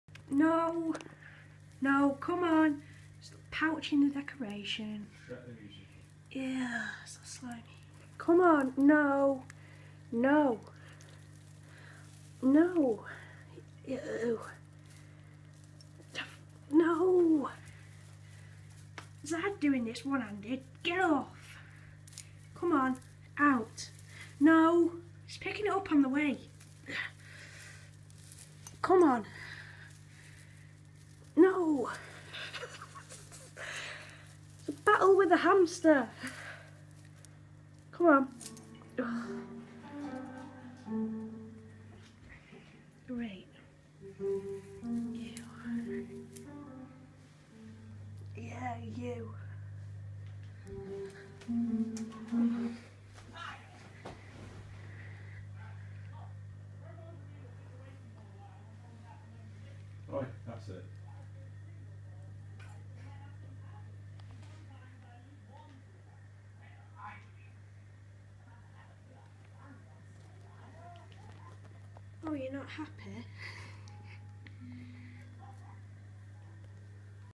Gary stealing decorations
He climbs up the Christmas tree and tries to steal the decorations. He loves hoarding things he has stole pens , inhalers , tiny bottles , tiny remotes , the golden wire tree and decorations off the tree . He tries to take them behind the tv to his collection of things . We let him roam around in the back room when he comes out